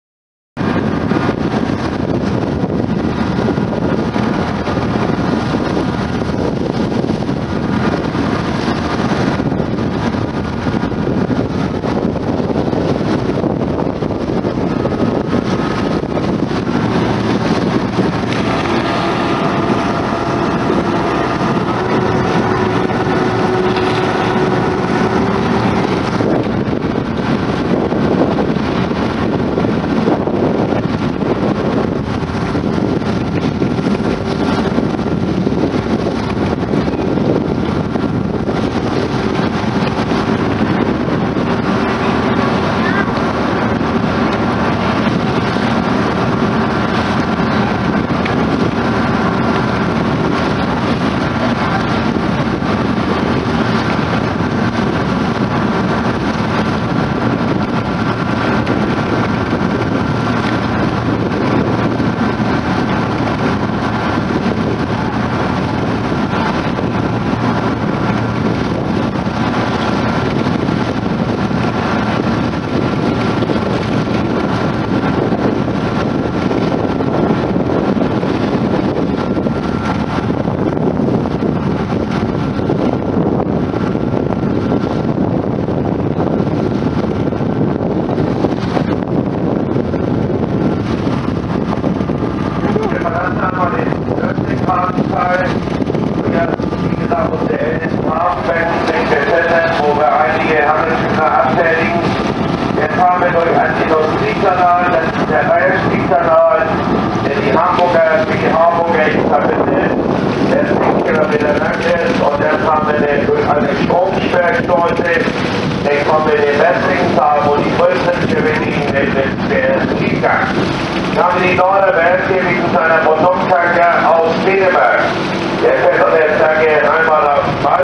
Hafenrundfahrt Hamburg, Deutschland (1/3) / Гавань Тур Гамбург, Германия (1/3)
Hafenrundfahrt in Hamburg, Deutschland durch die Speicherstadt und dann vorbei an den Kontorhäusern, der neuen großen Elbphilharmonie, der Universität, den Kreuzfahrt - Terminals.

Harbor tour in Hamburg, Germany through the Speicherstadt and then past the Kontorhäusern, the new large Elbphilharmonie, the university, the cruise ship terminals.

Гастроли в Гамбурге, Германия через Шпейхерштадт, а затем мимо Конторхаузерна, нового большого Эльбфилгармония, университета, терминалов круизных судов.